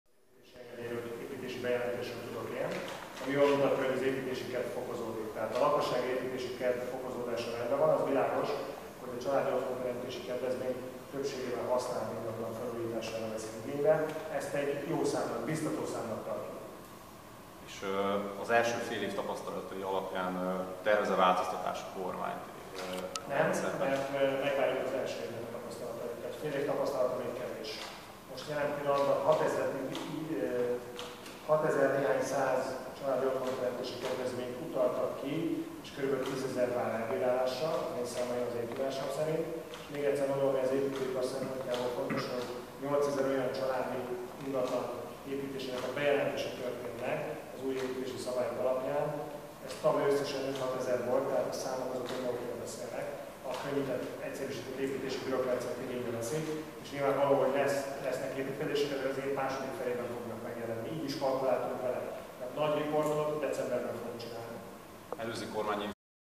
56. Kormányinfó - Lázár János .- CSOK felülvizsgálat ... 15.
Fokozódik a lakossági építési kedv ... ezért sem várható változás a CSOK-ban. Egy év után vizsgáljuk meg, értékeljük a tapasztalatokat ...